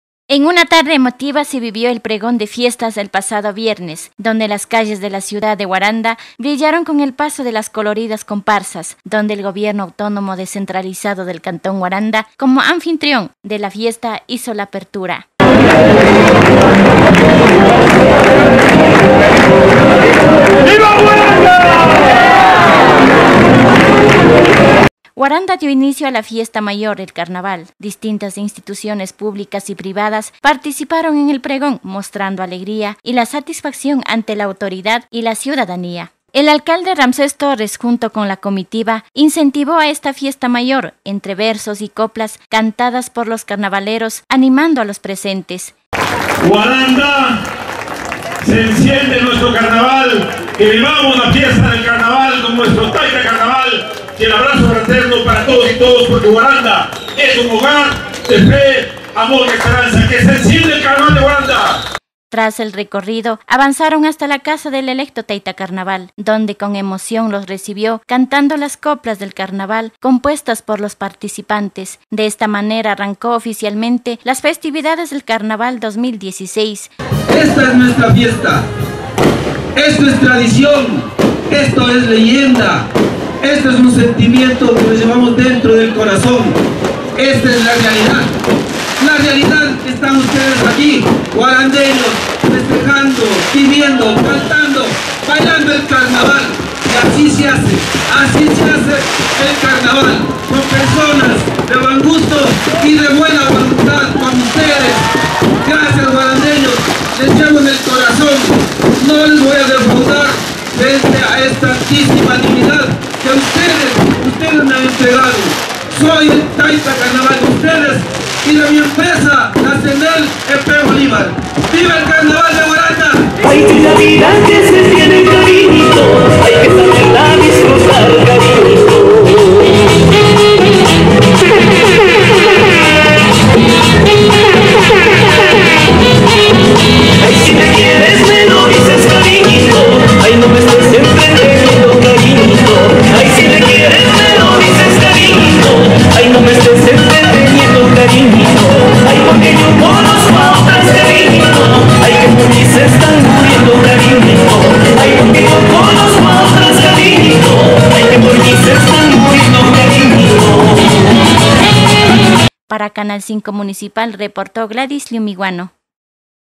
PREGÓN DE FIESTAS DEL CARNAVAL 2016.
Con un emotivo acto inicio el pregón de fiestas de los carnavales 2016.